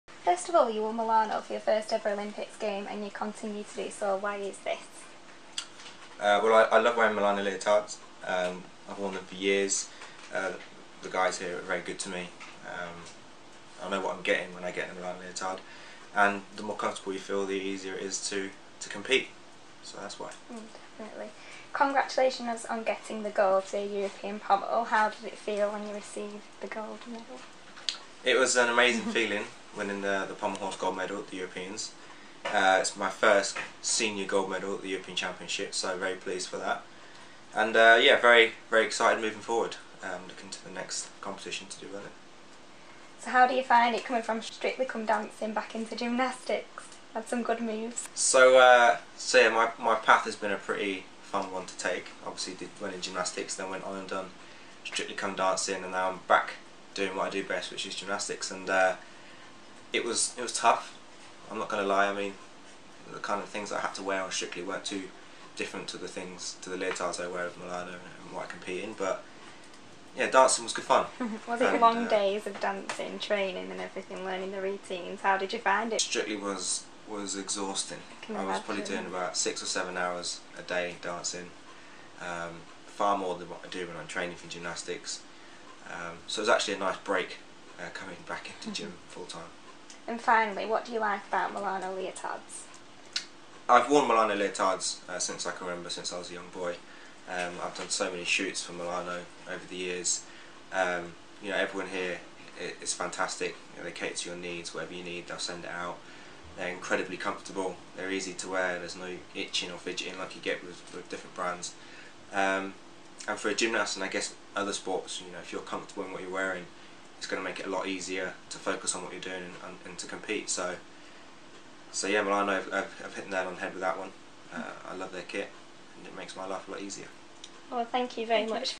Behind the scenes @ Milano Pro Sport - Louis Smith
On Monday 11th May 2015 we interviewed Louis Smith at Milano after the photo shoot for our new summer collection.